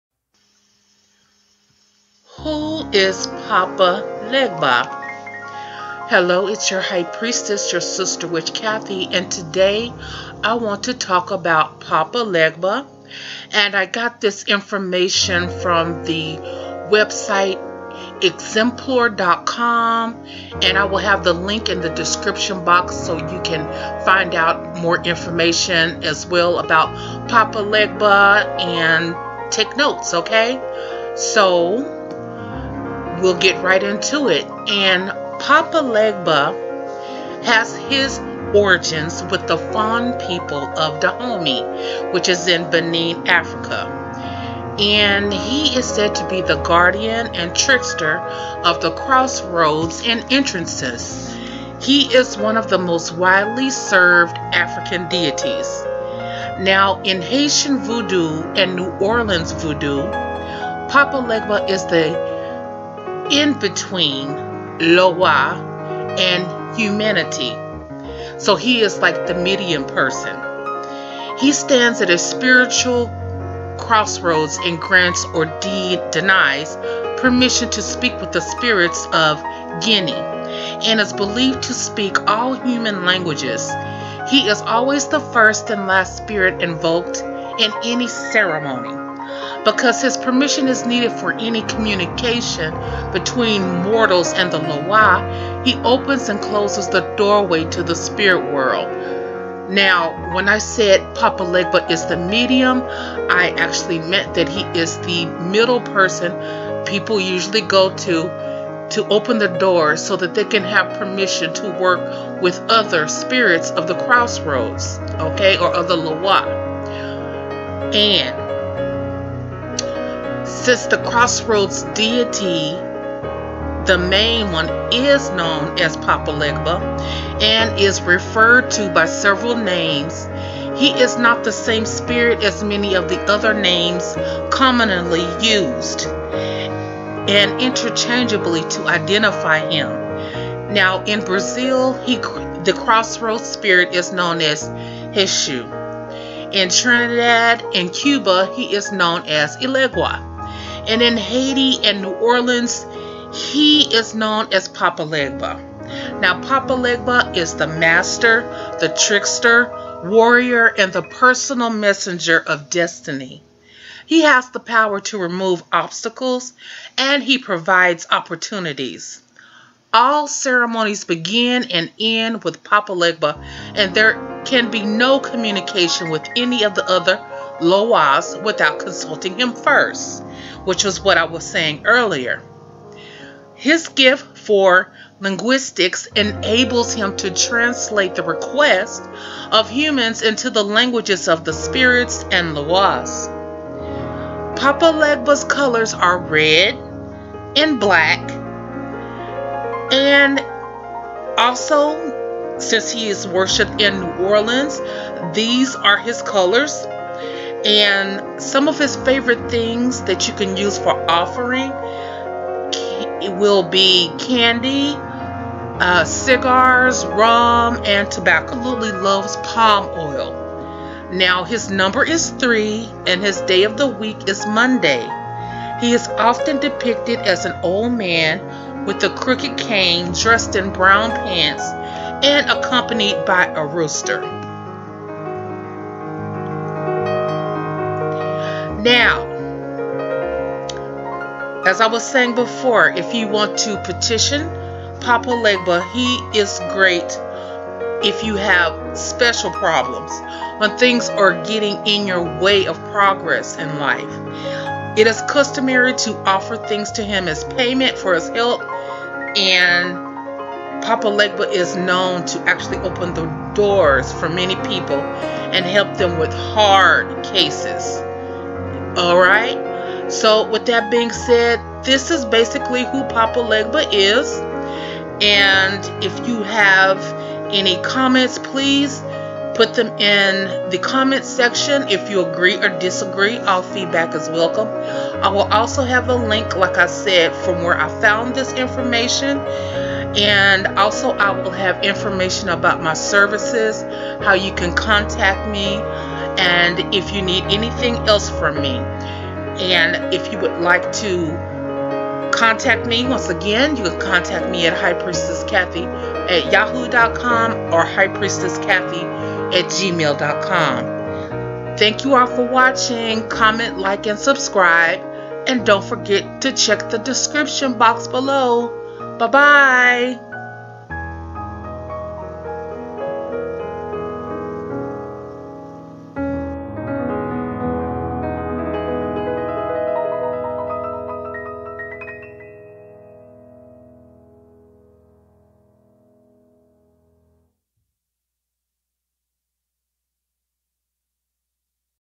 Who is Papa Legba?
Who is Papa Legba? Listen to this video while I explain who is and what he likes as offerings. I do spirit readings, hoodoo, rootwork and spellcastings.